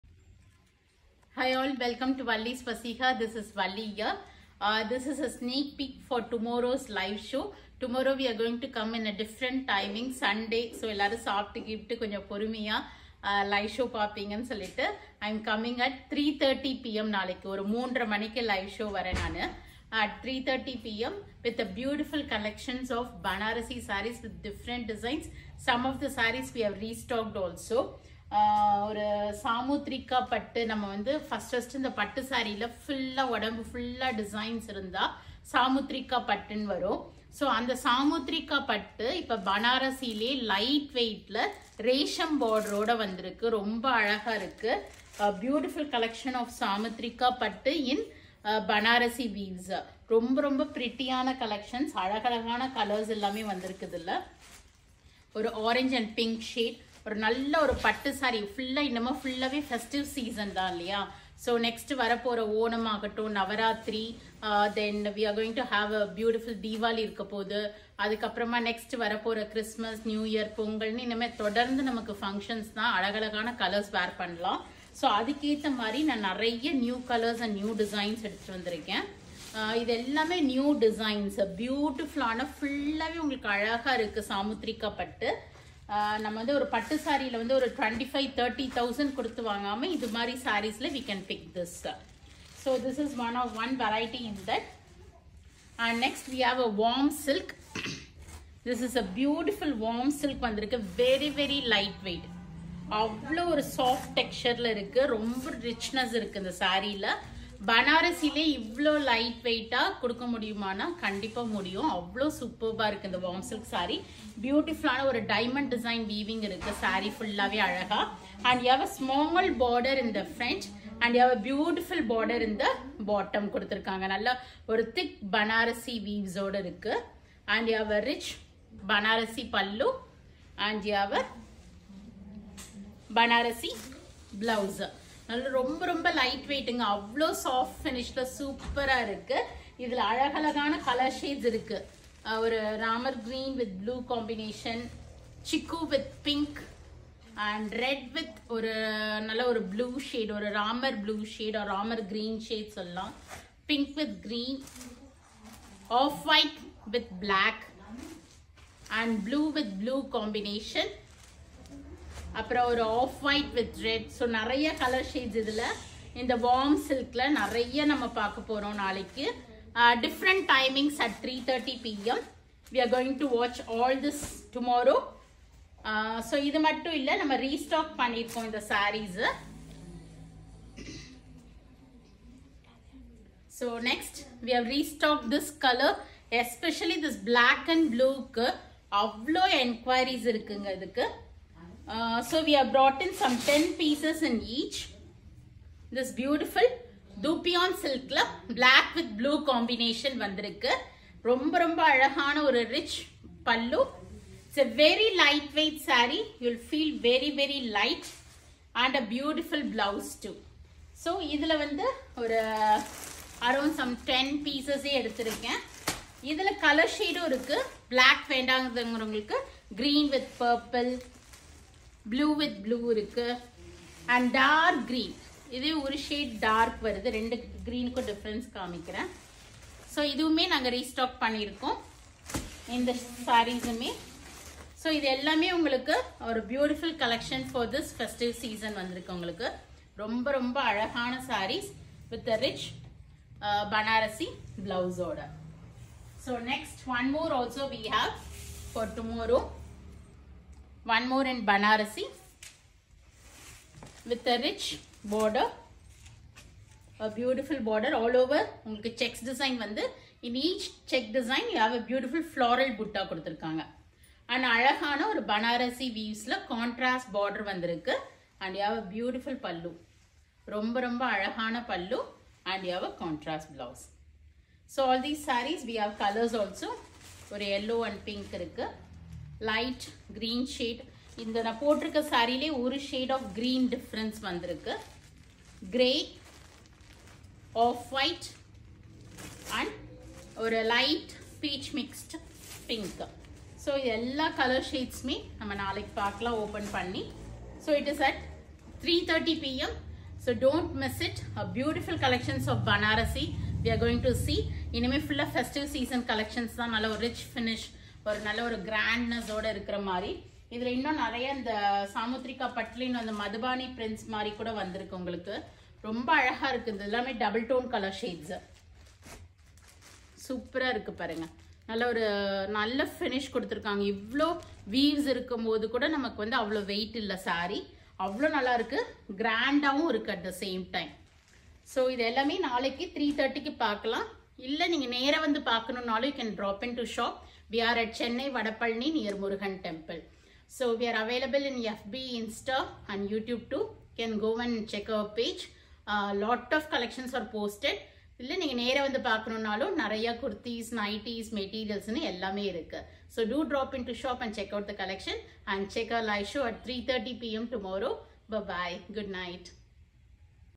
Sneak peek for tomorrow’s live show at 3:30pm ( 4/9/22)
Exclusive Samuthrika pattu in banarasi weaves ... Semi Dupion silk  and Warm silk sarees...